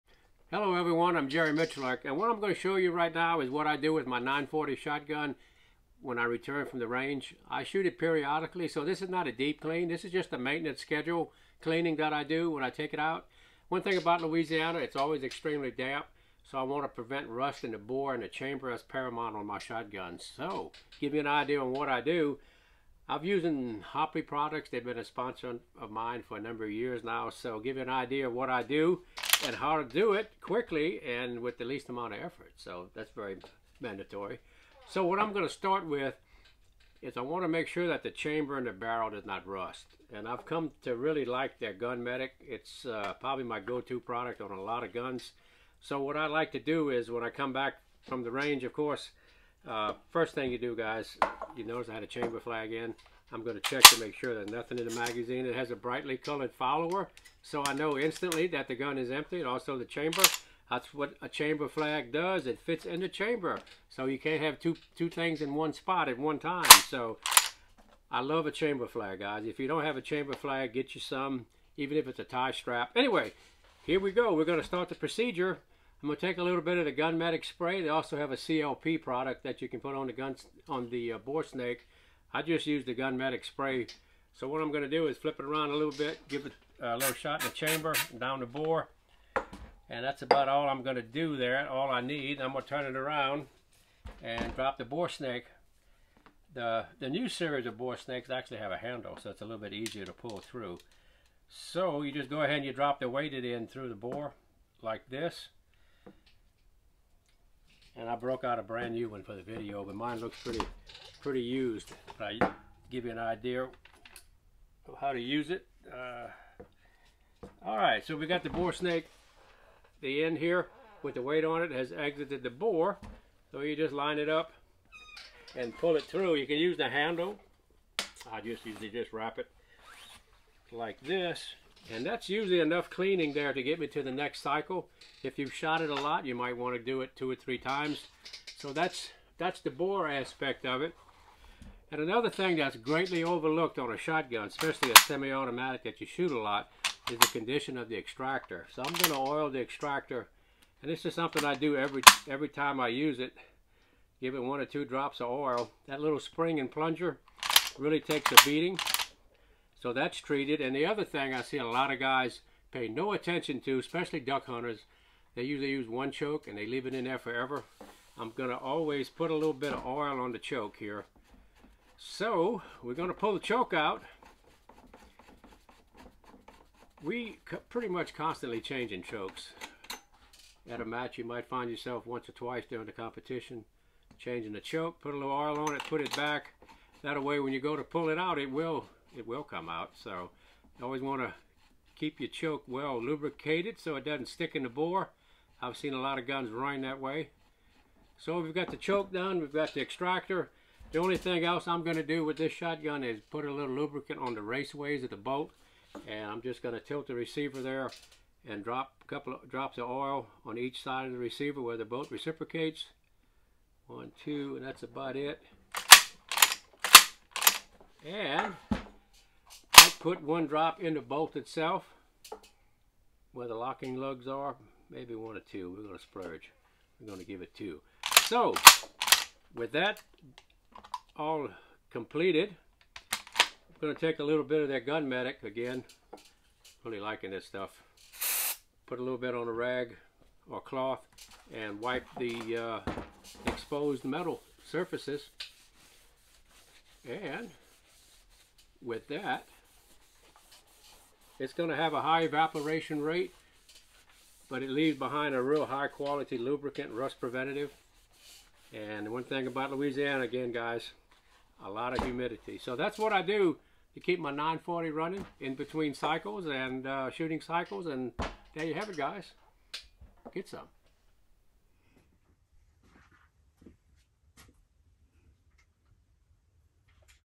A Little Shotgun Maintenance
What I do to maintain my Mossberg 940 JM Pro, or any other semi auto shotgun. Quick and simple!